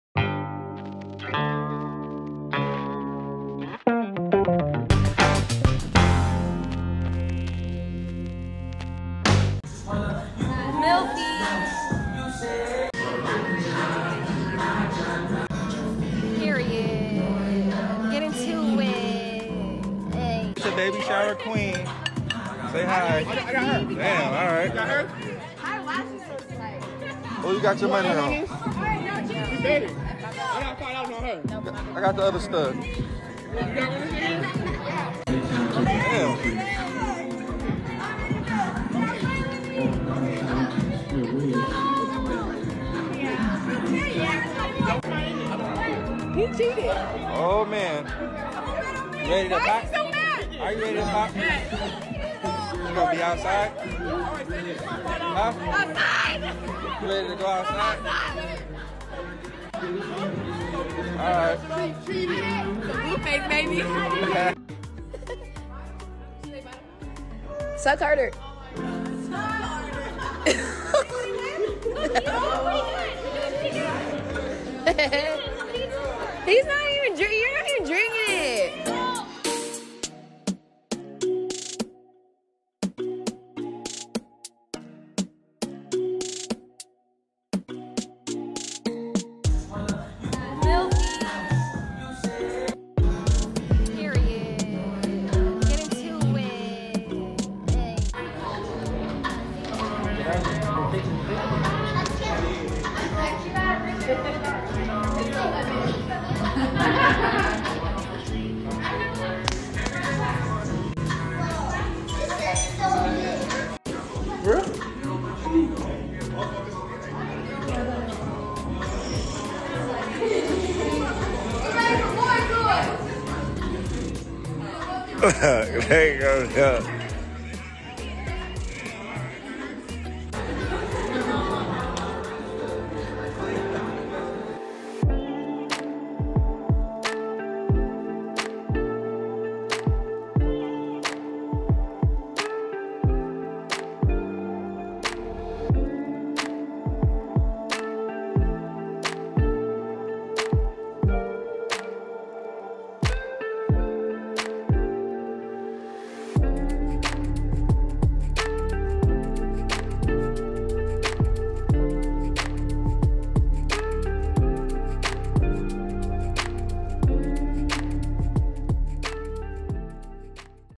Blueface & Jaidyn Alexis Host A Beautiful Baby Shower For Their 2nd Child! 👼🏽
►Blueface & Jaidyn Alexis enjoyed a beautiful baby shower with family and friends in honor of the impending arrival of their baby girl.